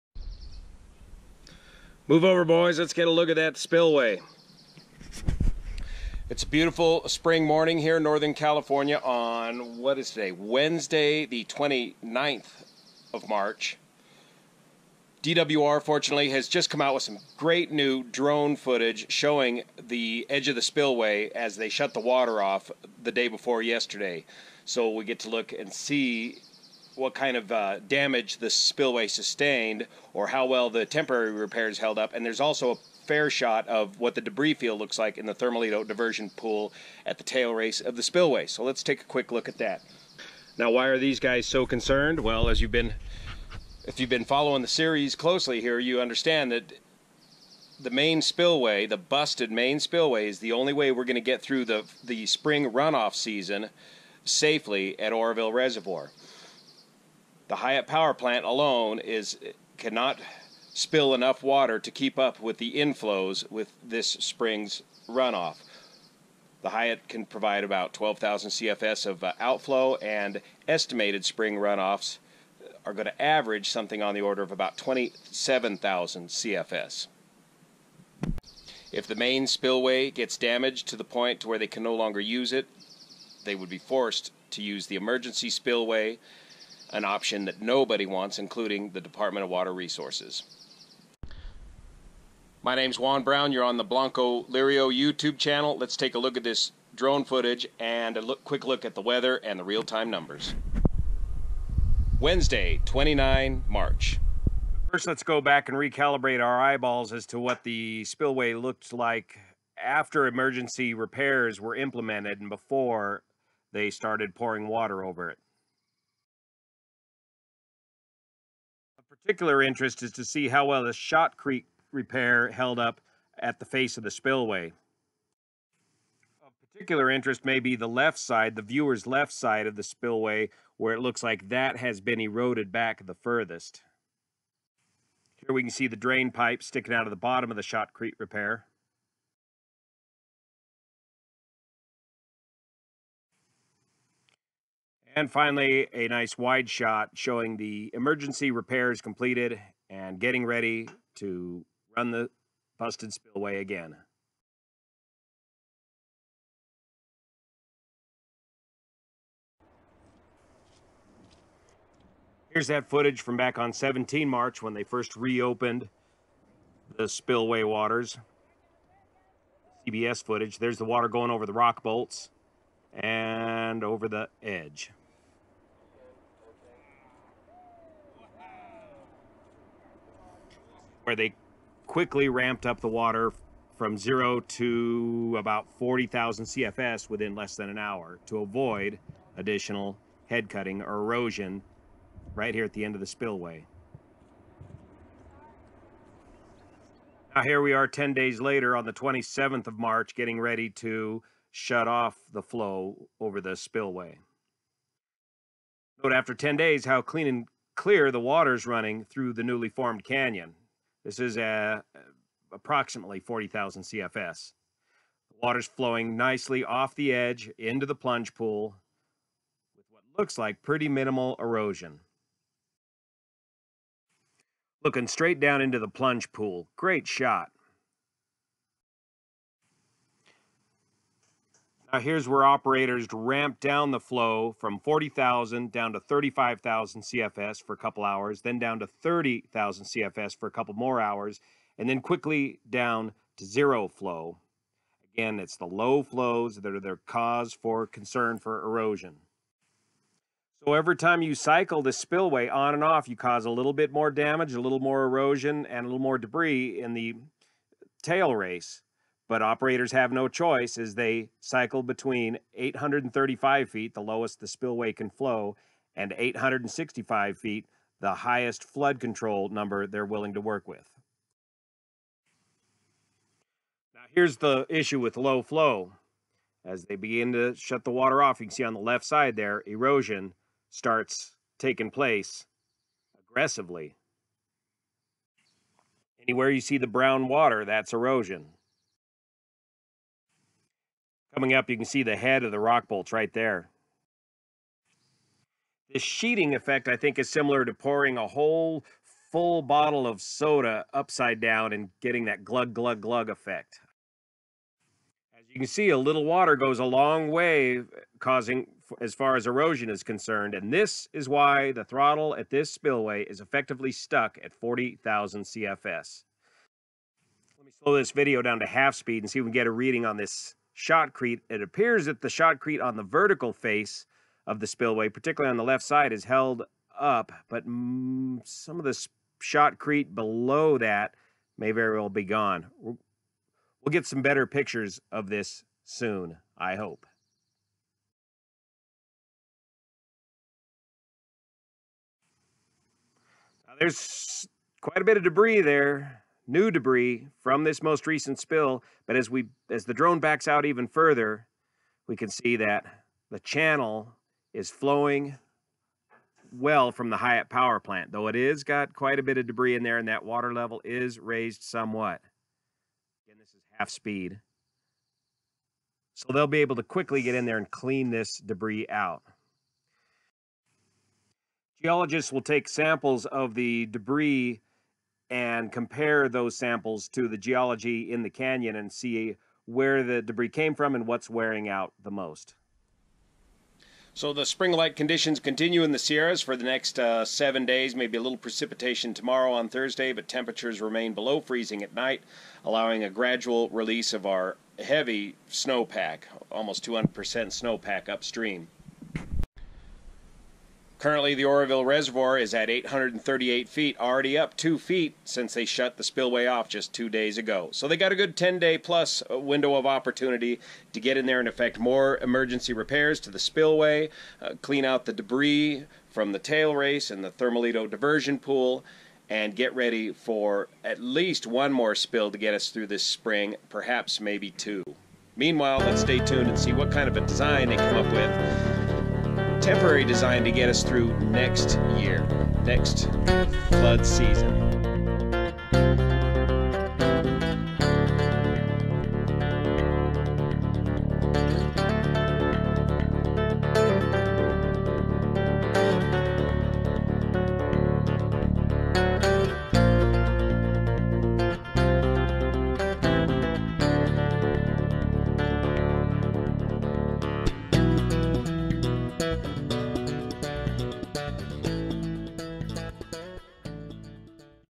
Oroville 29 March New DWR Spillway Footage
Take a preliminary look at how the spillway fared after the most recent cycle.